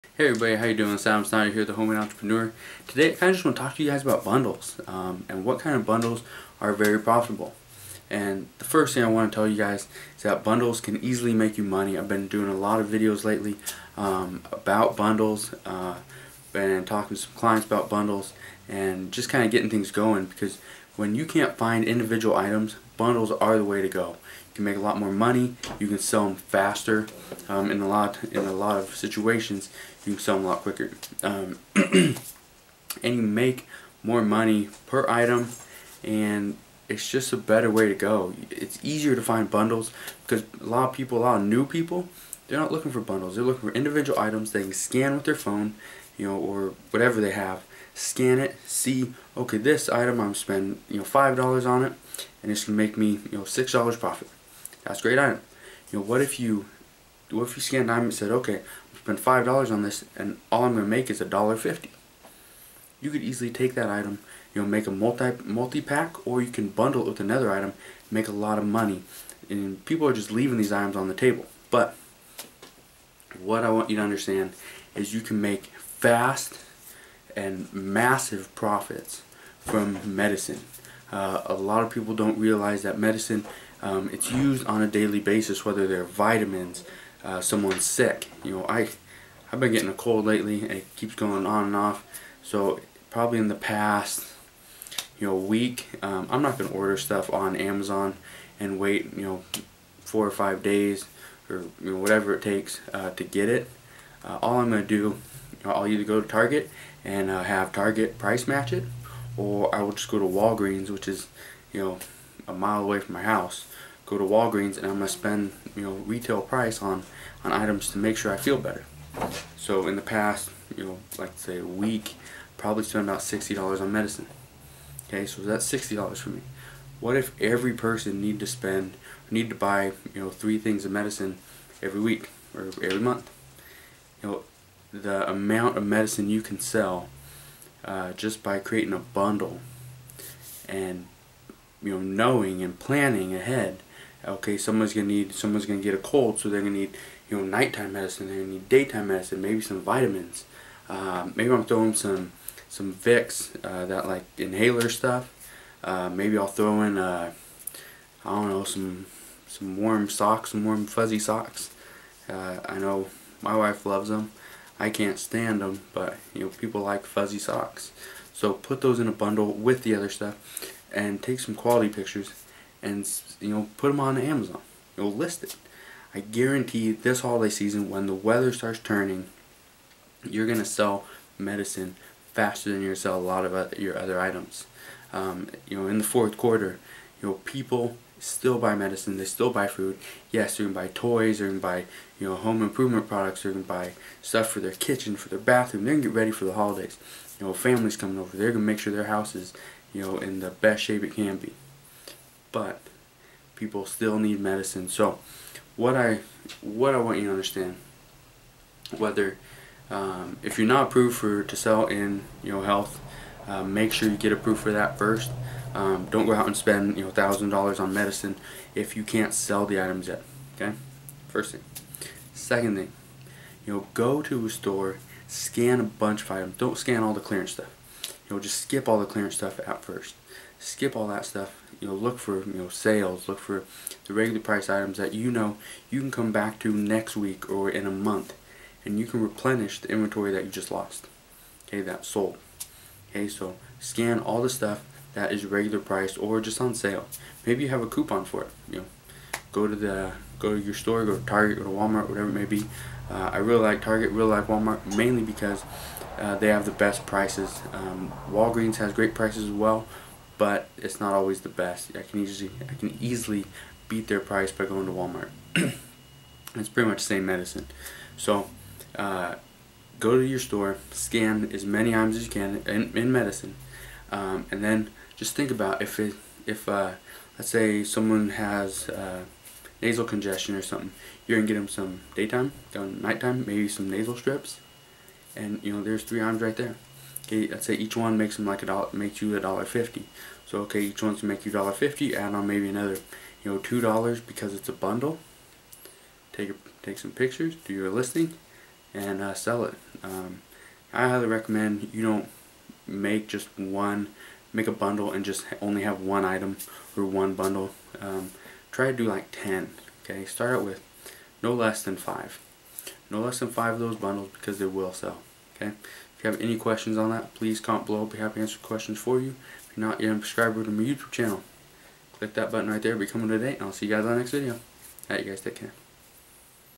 Quick $3,000 - Amazon FBA Medicine Bundles - Fast MASSIVE Profits
Making money quickly is what everybody wants to do, but that isn't always possible. You don't have to be a professional athlete to cash in  on something, you just have to think a little bit differently. This is what I have been doing.

I have been taking medicine and putting it into bundles. This will give me more products to sell and it also gives me a higher profit margin per item.